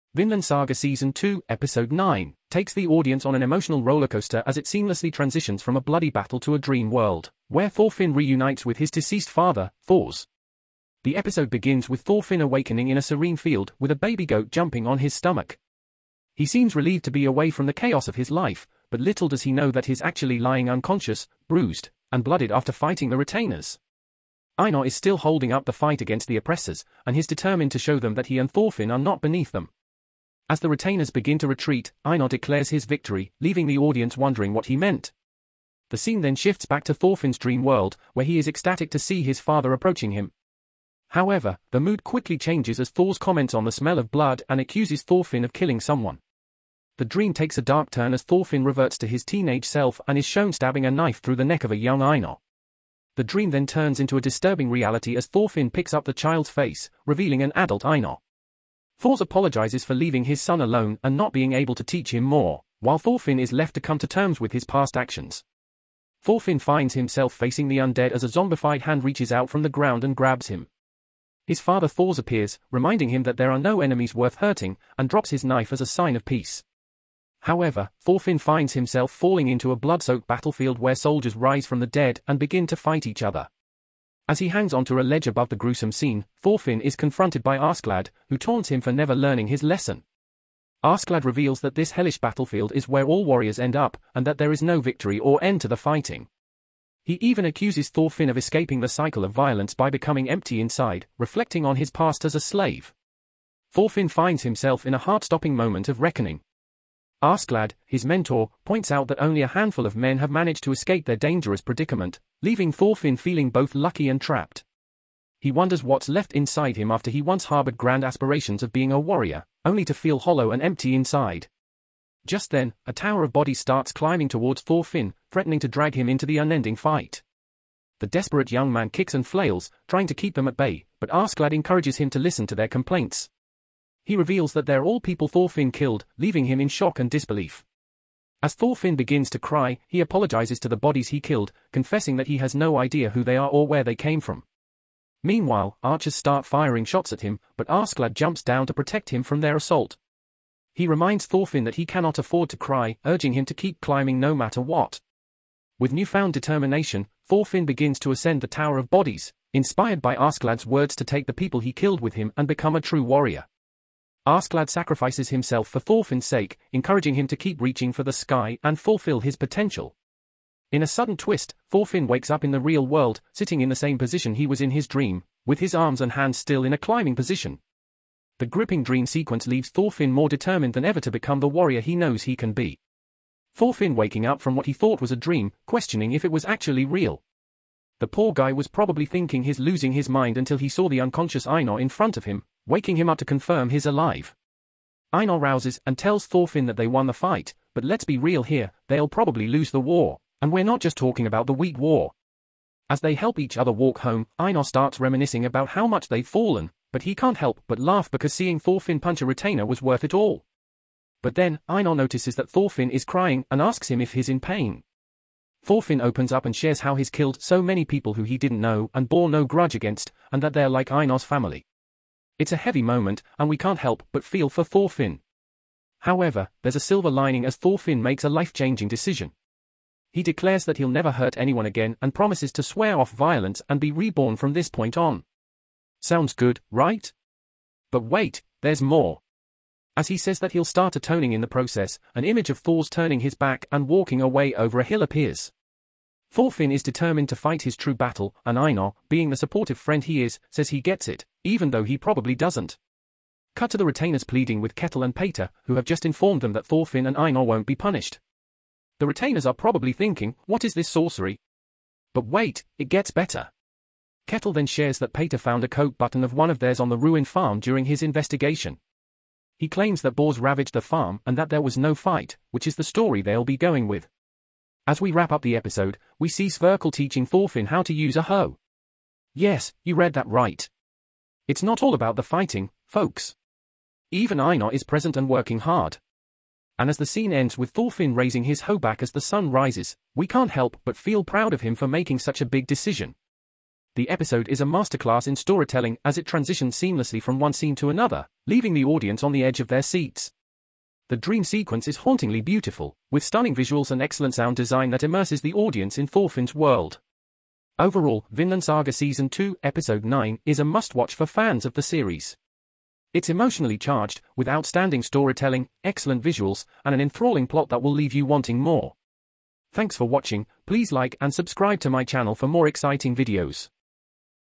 Oath | Vinland Saga Season 2 Episode 9 Review 🔥🔥
Vinland Saga Season 2 Episode 9 | Oath | Review & Reaction

Vinland Saga season 2 episode 9 takes the audience on a dream-like journey where Thorfinn reunites with his father, Thors. As Thorfinn wakes up from a serene dream, he finds out he has been fighting with the retainers and is bloodied and bruised. Meanwhile, Einar continues fighting against the oppressors and declares his victory. Thorfinn is then shown having a disturbing dream where he confronts his past and comes face to face with his victims. Askeladd taunts him for never learning his lesson and encourages him to climb the tower of bodies, which he had created. With newfound determination, Thorfinn starts ascending the tower of bodies, inspired by Askeladd's words, but Askeladd sacrifices himself for Thorfinn's sake. Thorfinn wakes up in the real world with newfound determination to become the warrior he knows he can be. He makes a life-changing decision to swear off violence and atone for his past actions.